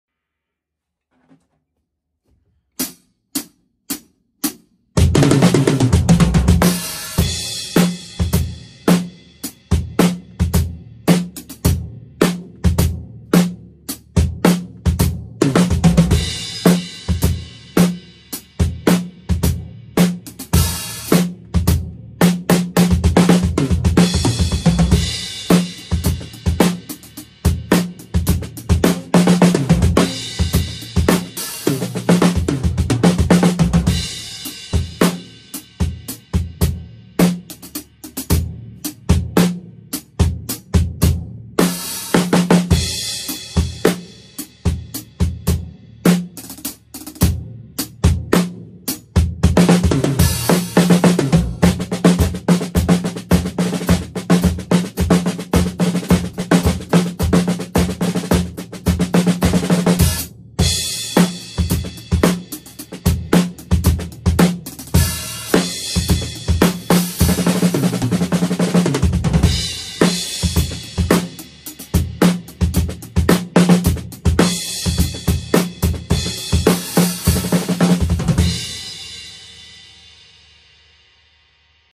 동원대 실용음악과 드럼제자 김근유 드럼솔로
다양한 컴비네이션과 리듬을 결합한 드럼솔로영상
연습 더 열심히 해서 앞으로 많은 발전이 있길 바란다 ^^
나랑 다양한 장르 도전해보자 ^^